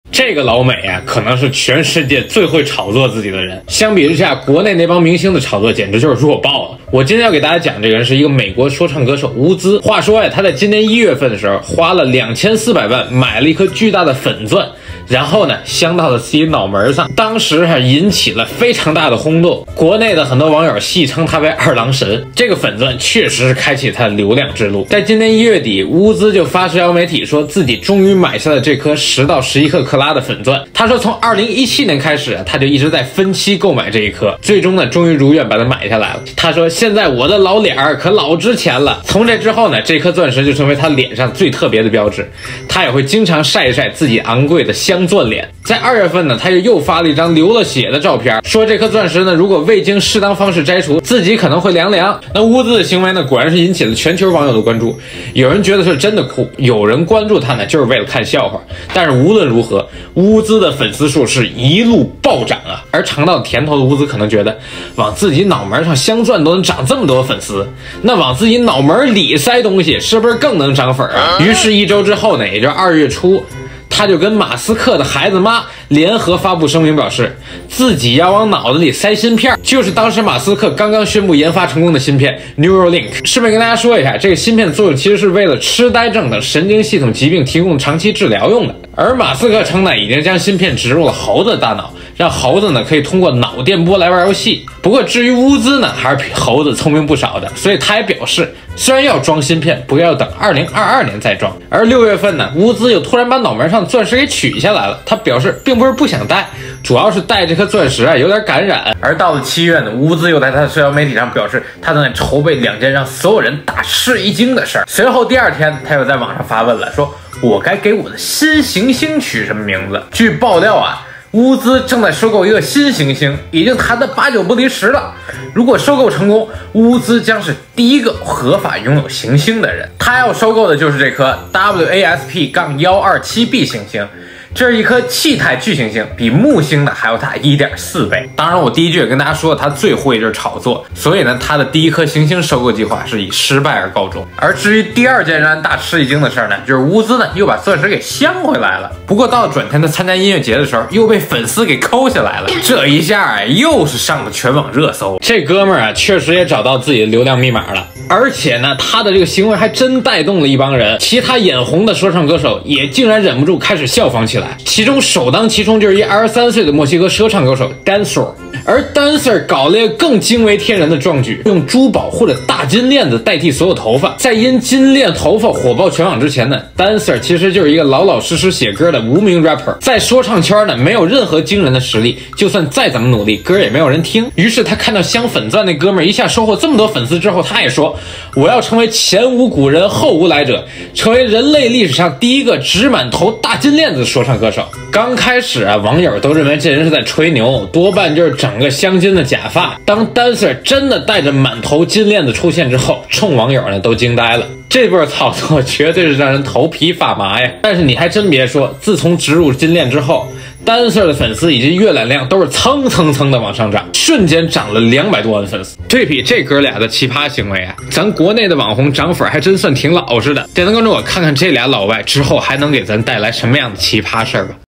本以为往脑袋上镶钻就已经很扯了，没想到其他国外网红为了红都疯了！终于理解为什么中国要整治不良网红风气了
本以为往脑袋上镶钻就已经很扯了，没想到其他国外网红为了红都疯了！终于理解为什么中国要整治不良网红风气了#网红